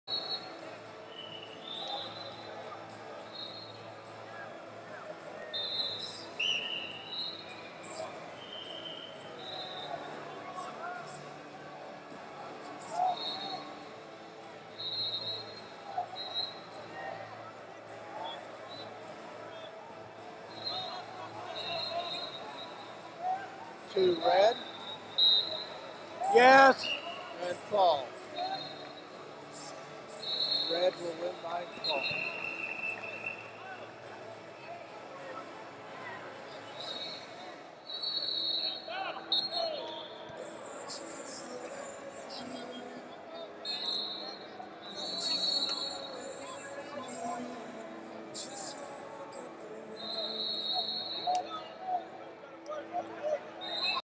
195 Lbs Rnd Of 64 Yahav Shraiber Wyoming Vs Brock Schilling Colorado
2019 USMC USAW Junior And Cadet National Championships - 195 Lbs Rnd Of 64 Yahav Shraiber Wyoming Vs Brock Schilling Colorado